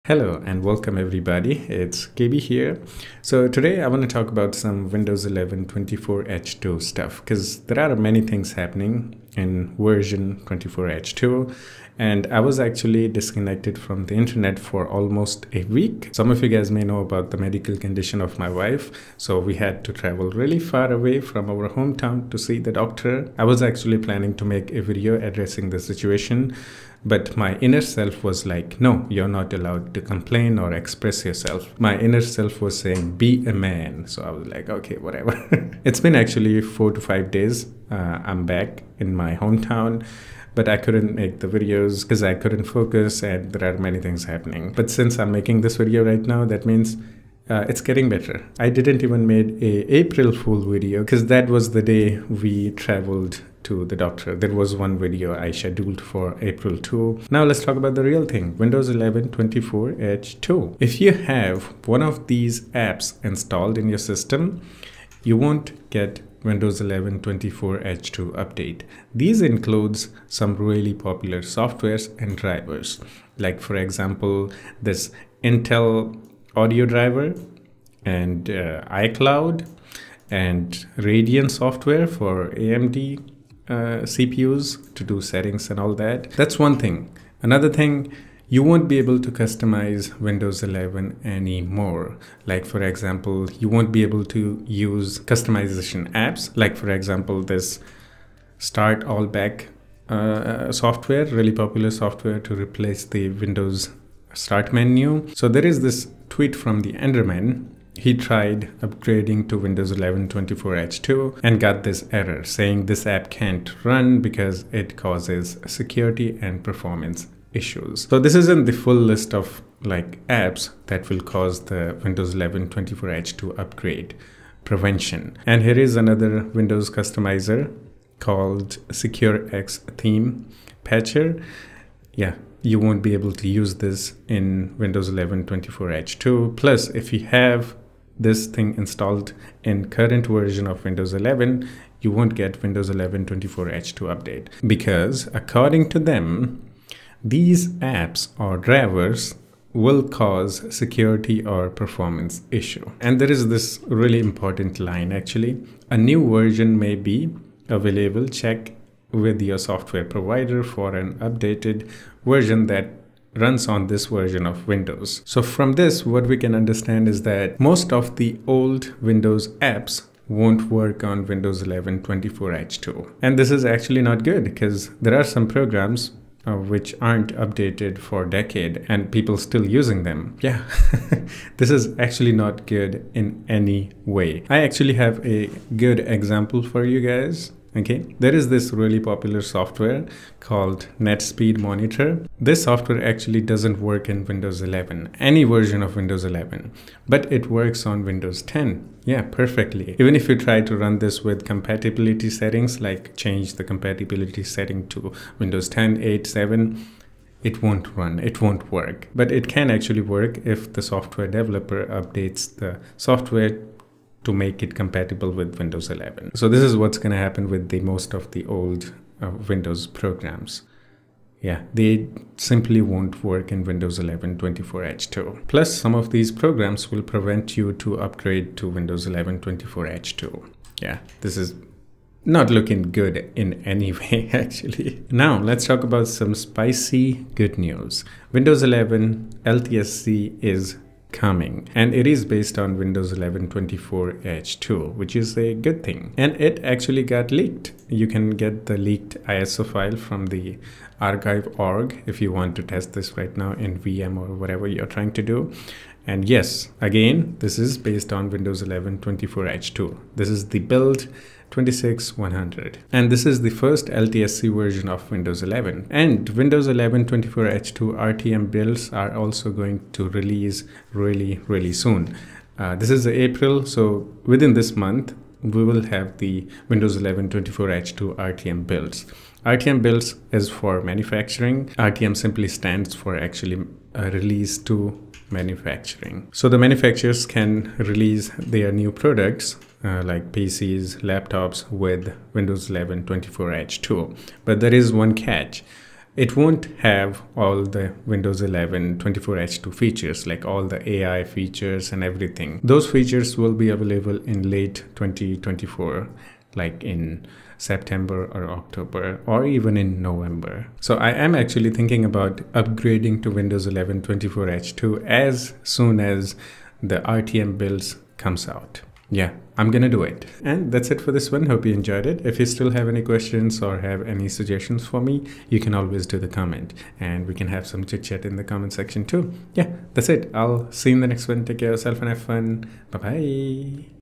You Won't Get Windows 11 24H2 Update If you have these programs installed
Windows 11 24H2 Update - you can't customize it, you can't run old programs.